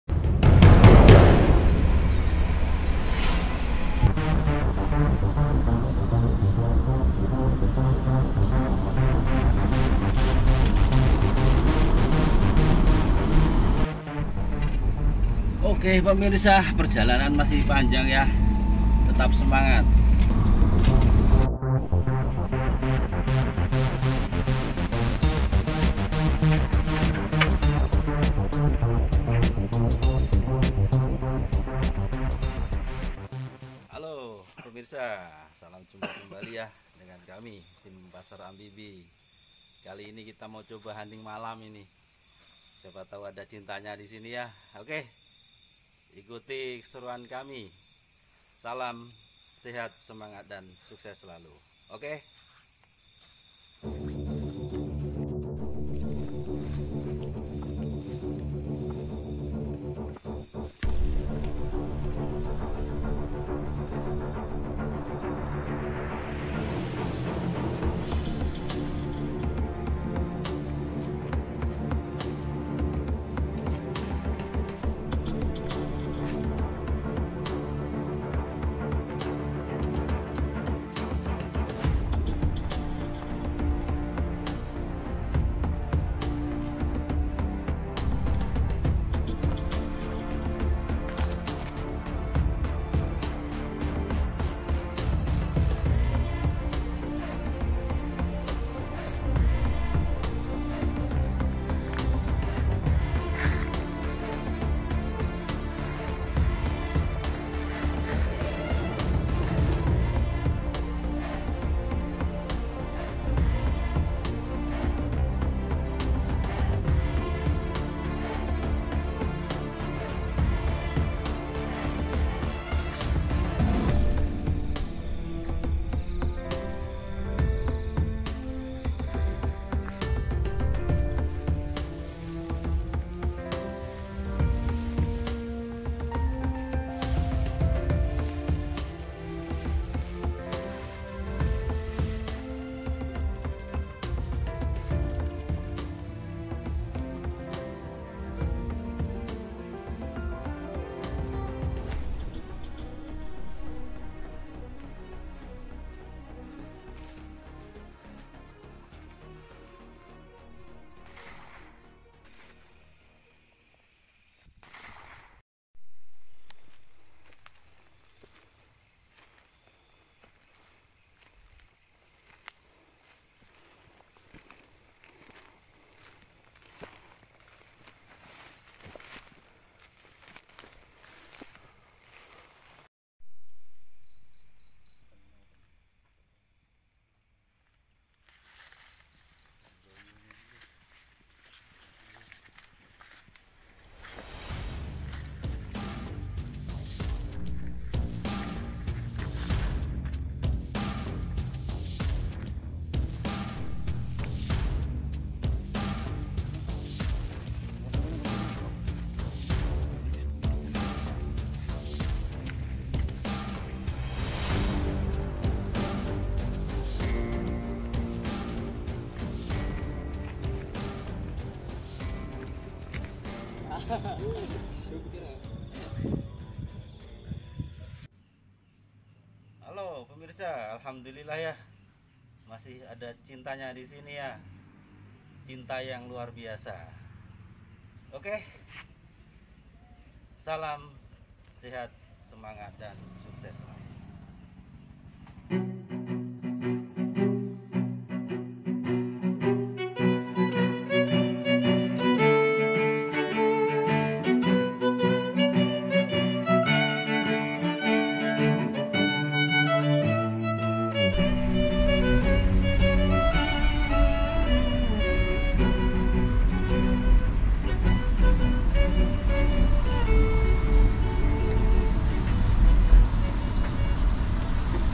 Berburu malam hari, Mbelor, Berburu Babi hutan
Salam jumpa kembali dgn tim Paser Ampibi 🙏
Kali ini berburu malam hari, Mbelor, berburu hama babi hutan, Alhamdulillah poin 2 ekor.
Mohon maaf tidak bisa menampilkan video yang lengkap dikarenakan keterbatasan peralatan.
Selamat menyaksikan keseruannya..,salam sehat, semangat dan sukses selalu.